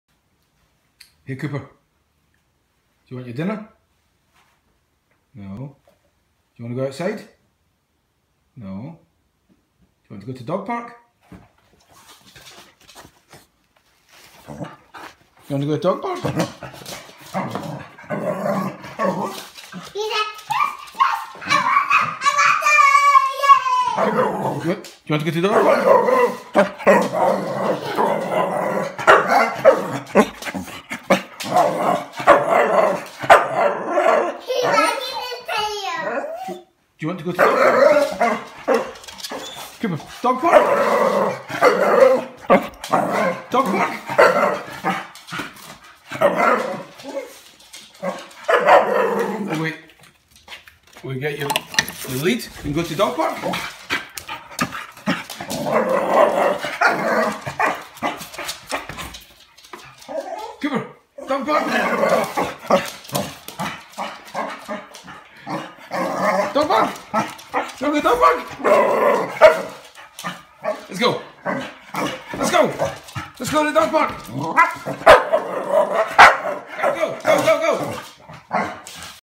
Cooper LOVES the dog park!
Cooper goes nuts at the very mention of the Dog park!